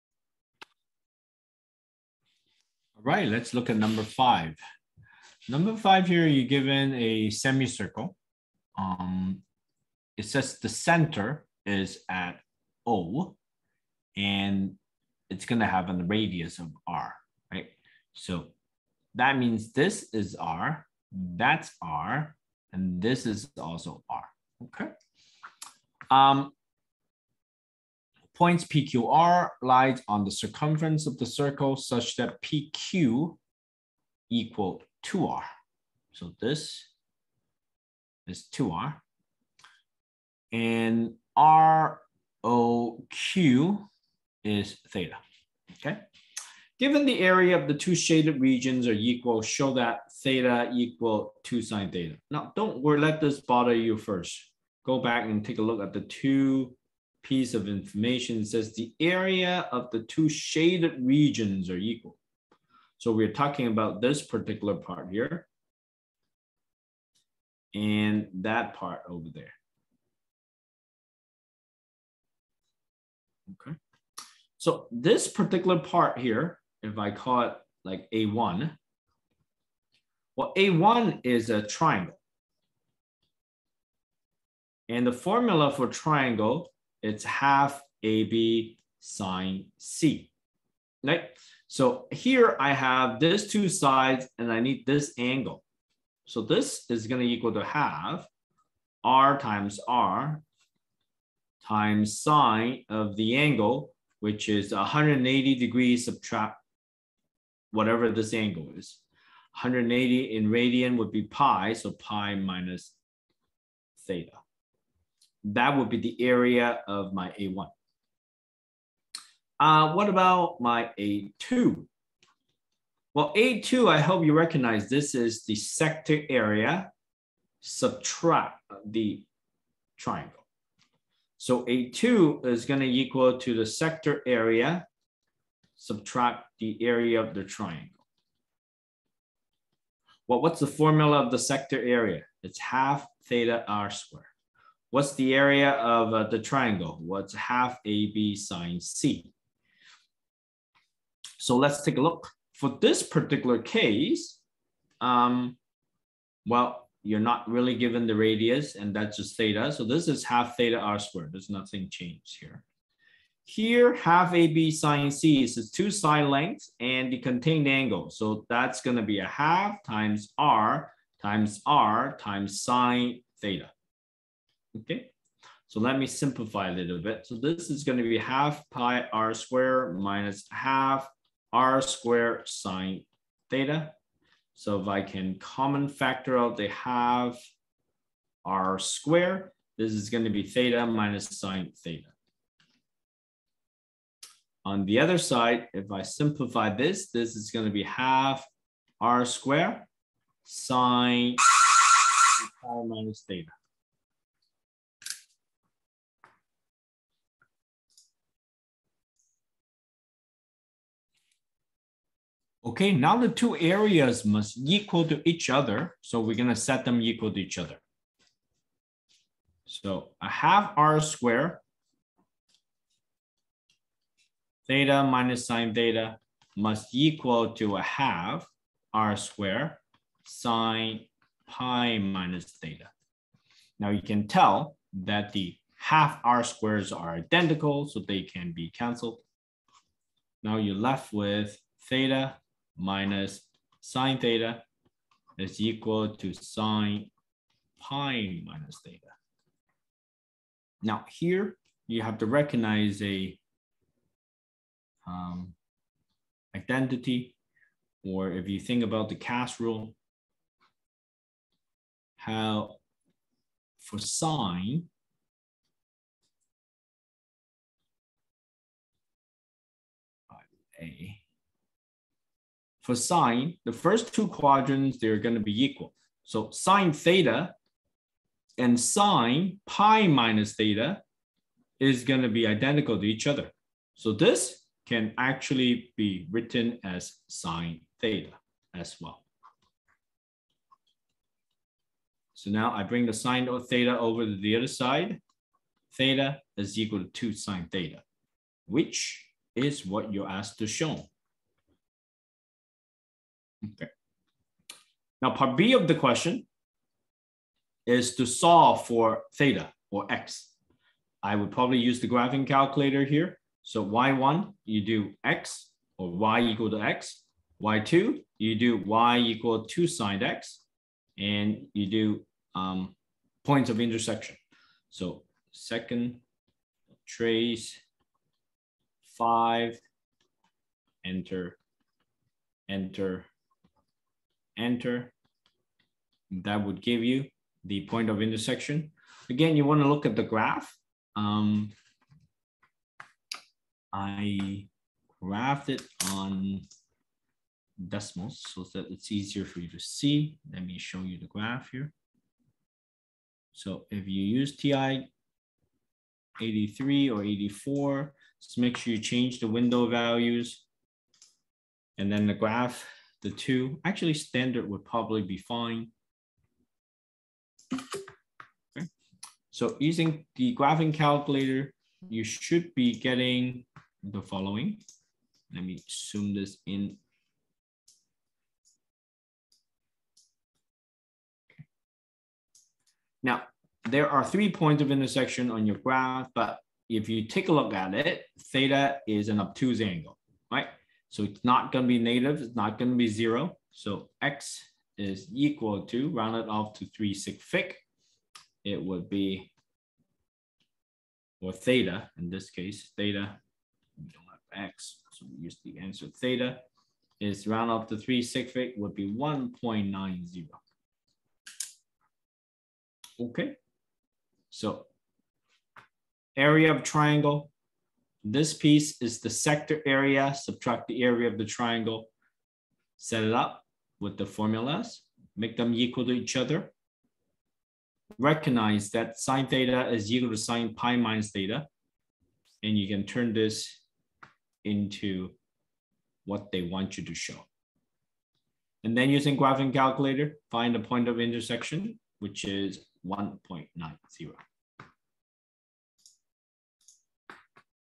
Nov21 IB Math AA SL Paper 2 #5
I'm going to take up the Nov 21 Math AA SL Paper 2.  Here's the solution to #5.